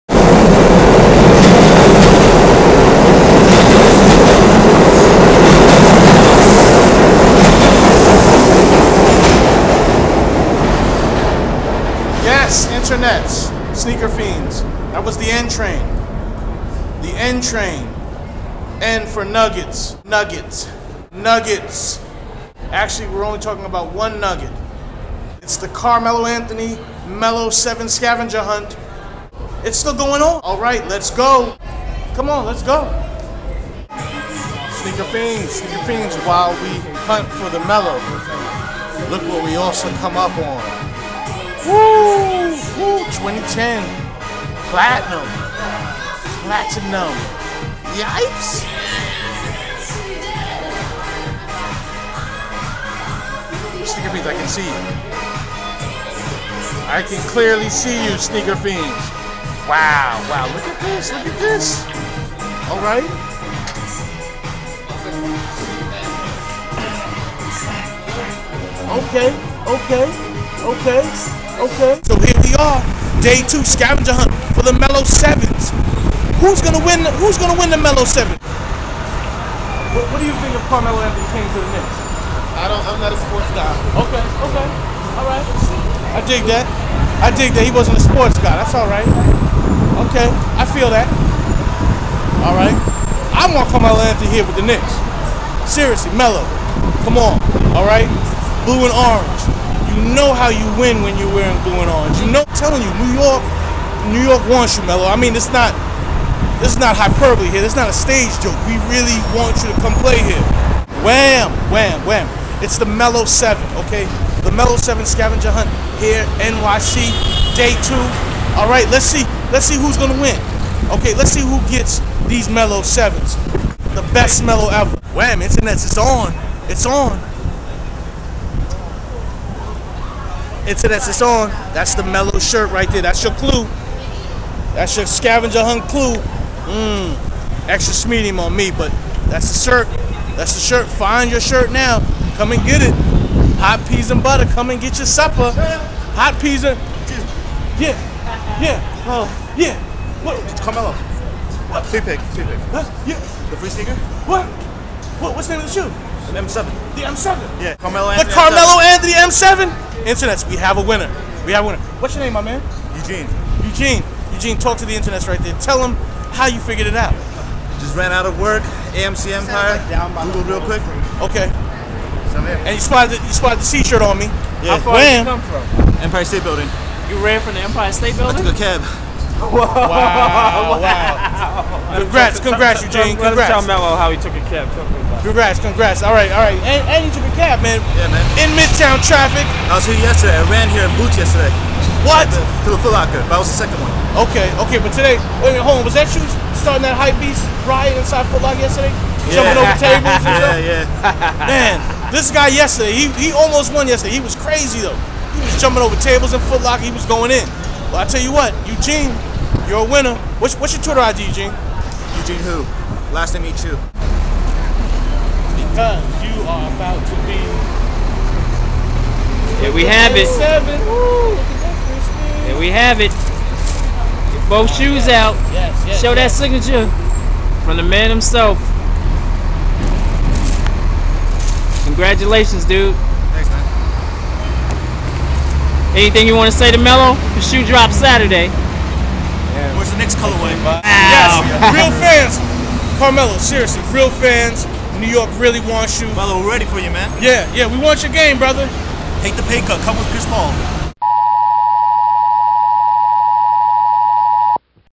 SNEAKER FIENDS UNITE! - MELO 7 SCAVENGER HUNT DAY 2
Day 2 of 3 in the Carmelo Anthony 'Melo7' scavenger hunt in NYC.

This pair is the white/black Chi-Towns.

Follow to get clues to find the next free pair to drop.

The Melo 7 drops in stores on Saturday November 20th.